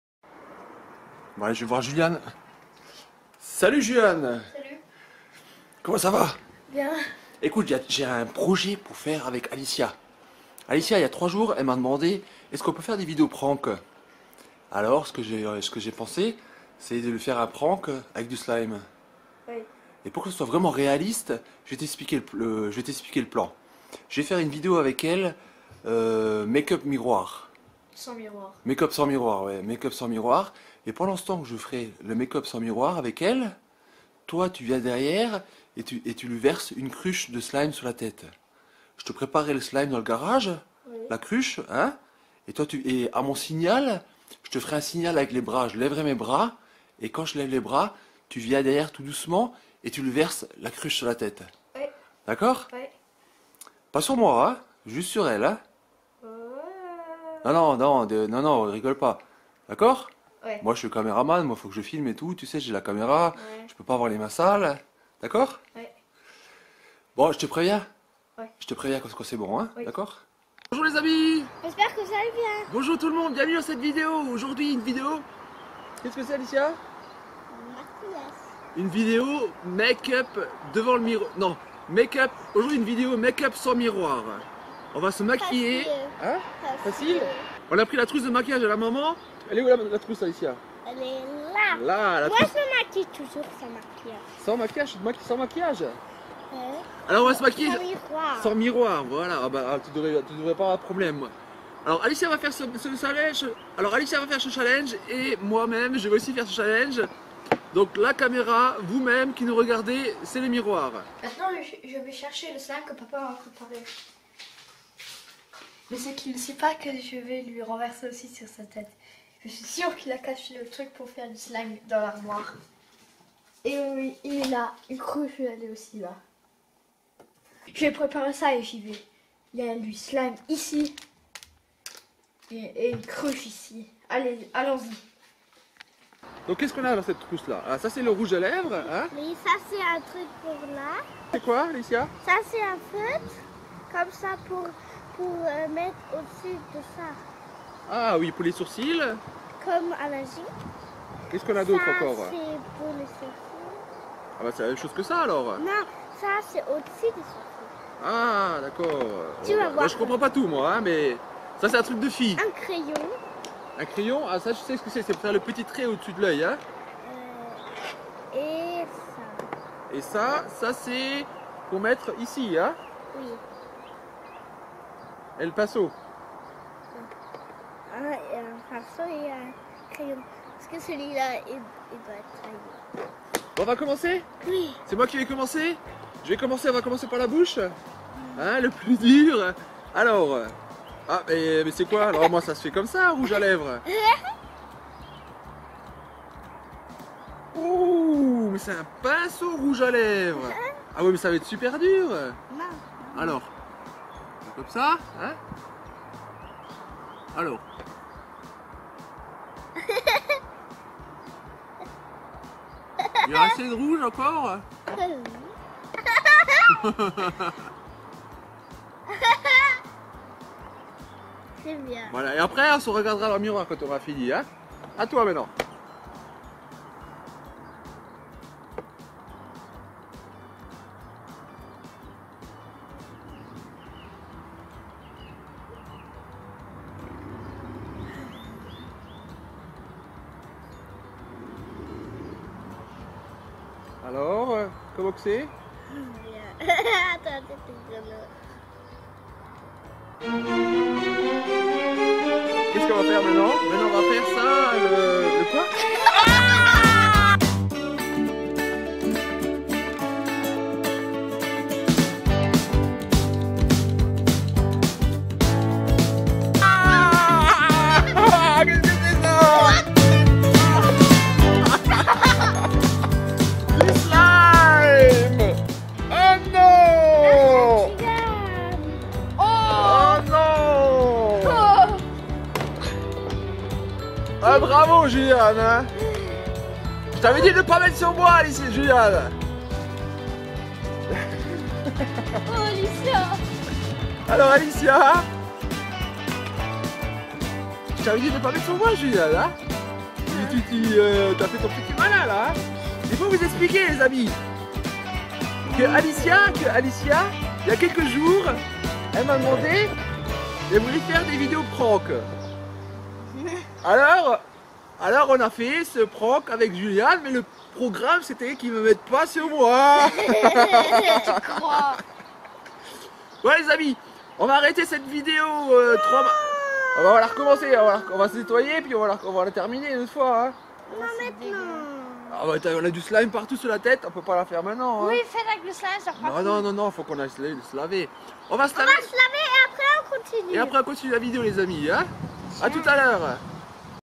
Julian renverse du SLIME sur Alicia pendant un tournage
SLIME BLAGUE sur Alicia et PAPA

Julian prepare une blague pour Alicia et papa avec  2 cruches de slime qu il va renverser sur eux pendant qu il tourne une video